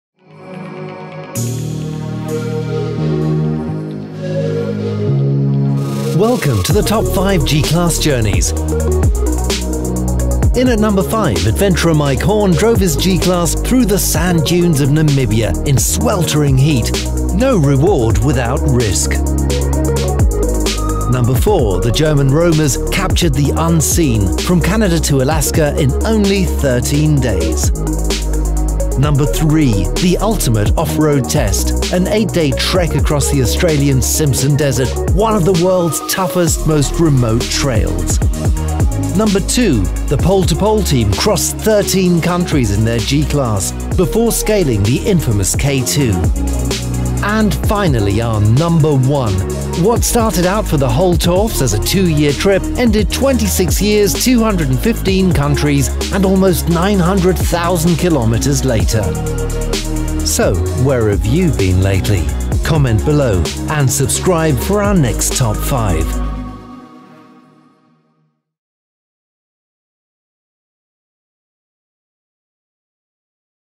Top 5 G-Class Journeys | Best of Benz
Welcome to "Best of Benz".  This episode is all about the most jaw-dropping corners of the world the G-Class has conquered. Join us on the off-road ride, and let’s get lost in some of the most wondrous places yet to be seen!

“Best of Benz” is regularly featuring the most surprising, entertaining or fun facts from the Mercedes-Benz universe.